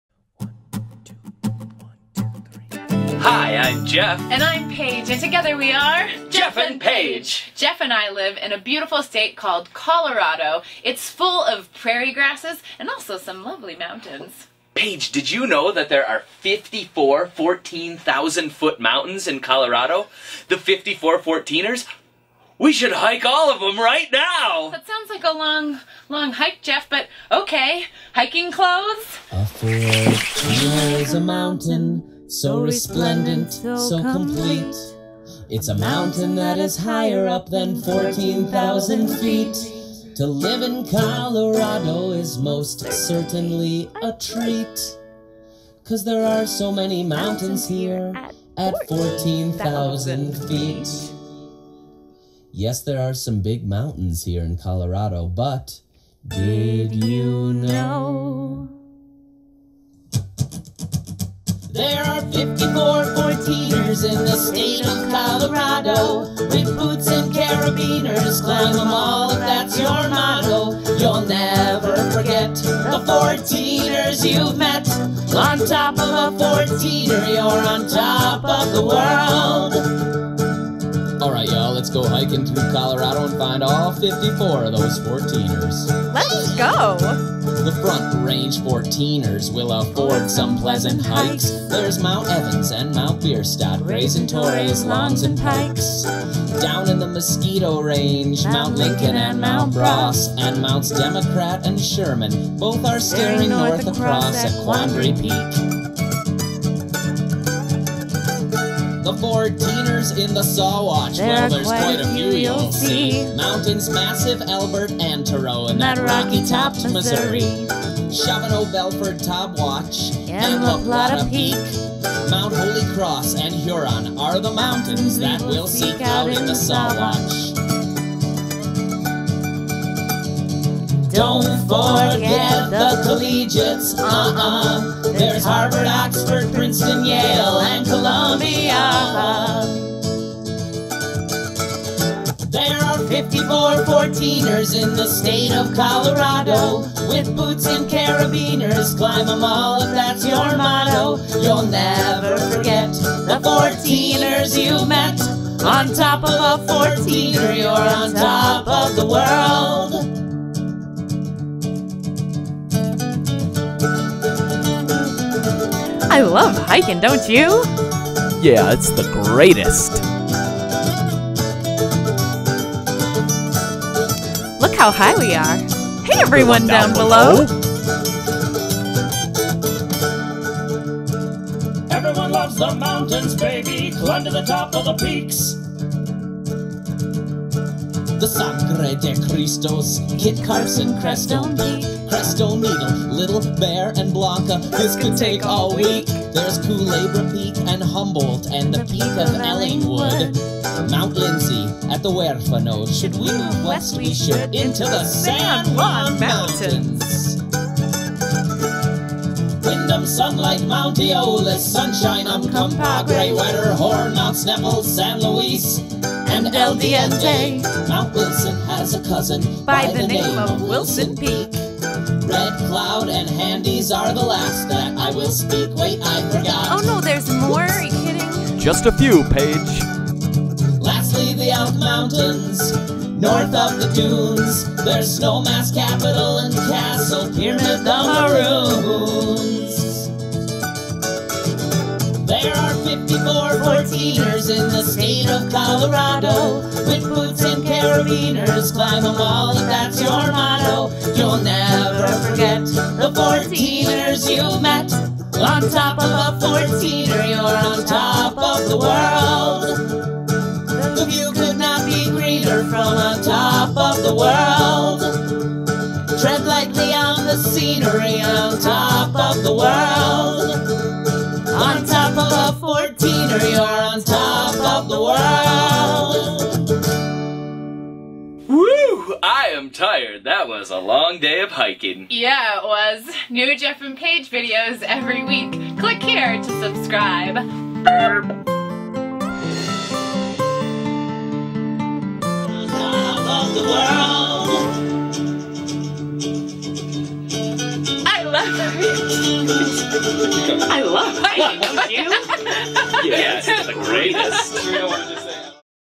54 Fourteeners
A  Fourteener is a mountain that rises over 14.000 feet above sea  level. (That's more than two and a half miles!) Jeff and Paige live in Colorado which is home to 54 of these amazing mountains and we're going to visit them all right now!

Photos: cc wikimedia commons & nps.gov